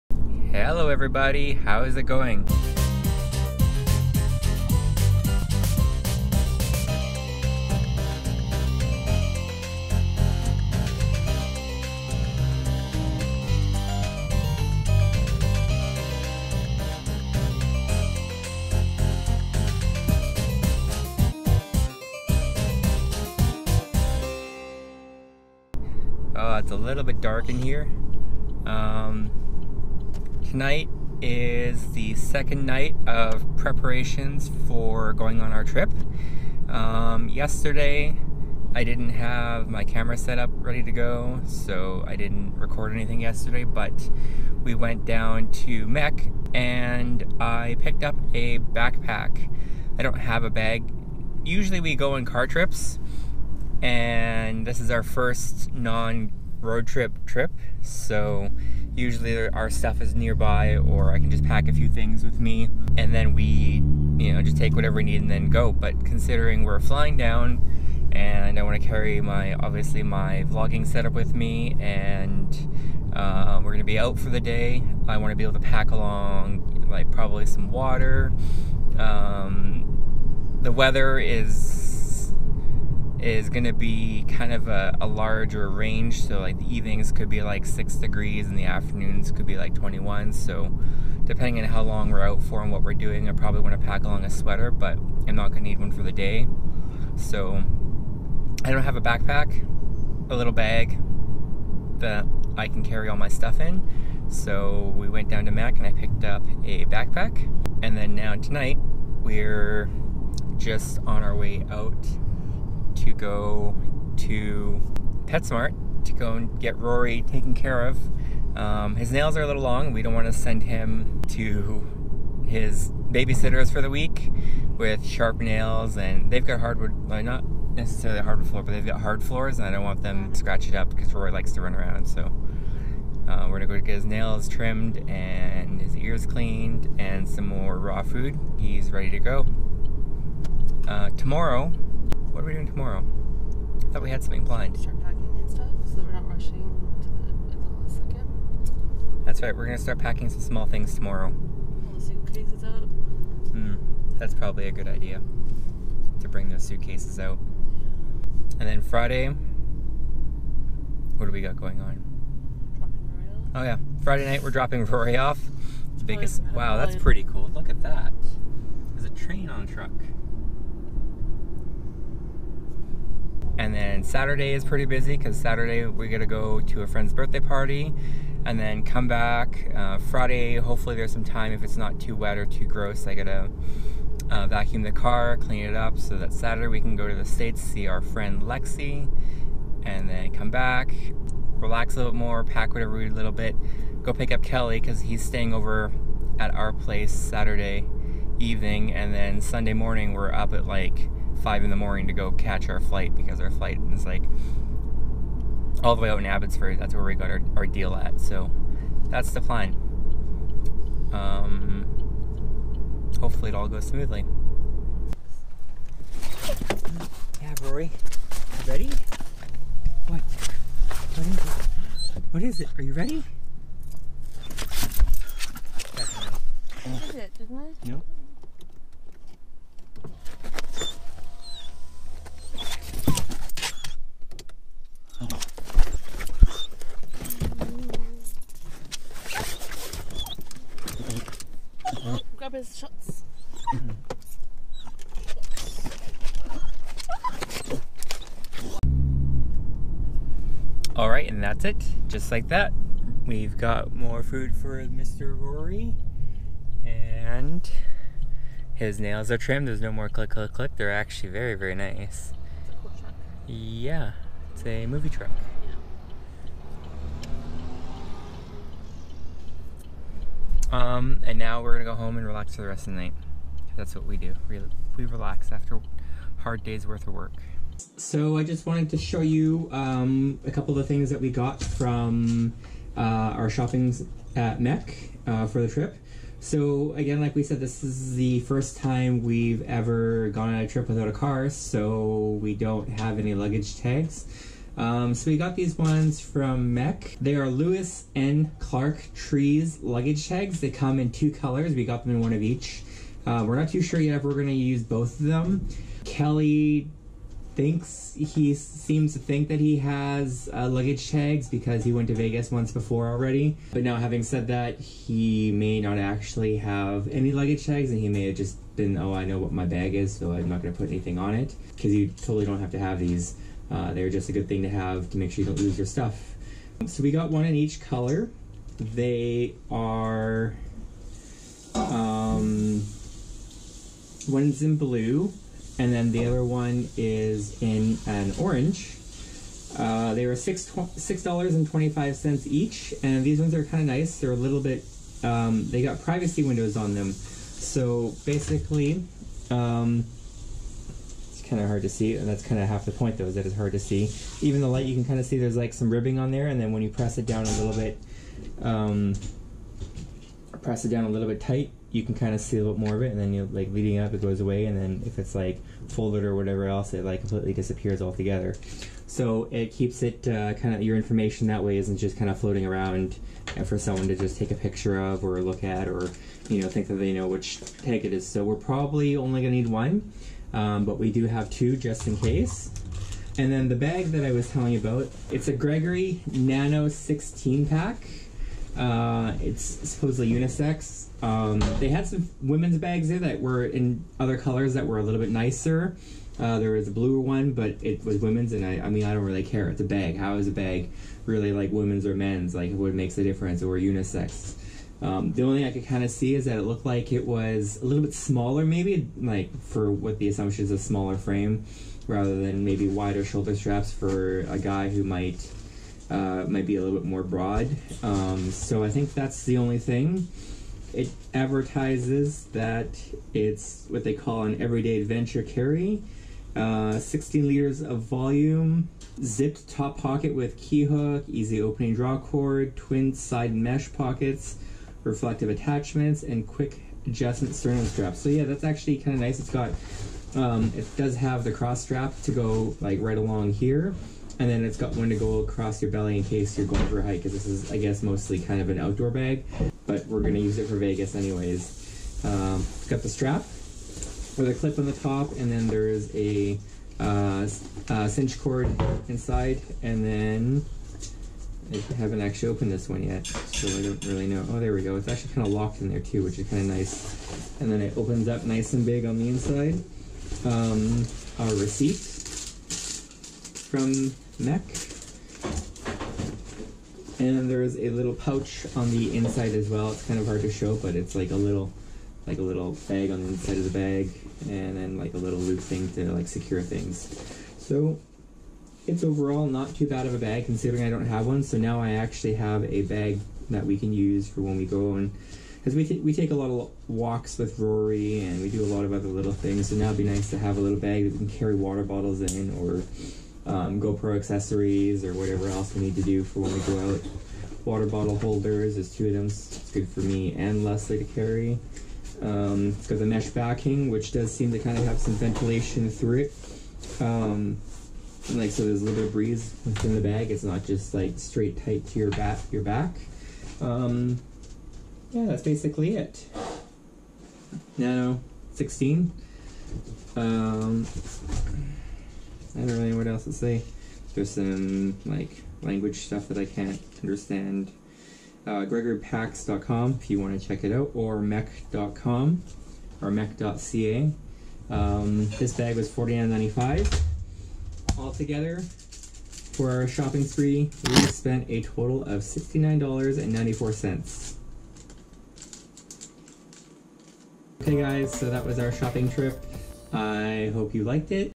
Jan 18&19 - Rory, and MEC haul
Getting ready for the trip. Rory gets a pawdicure at PetSmart, and we show you what we got from MEC for the trip to Vegas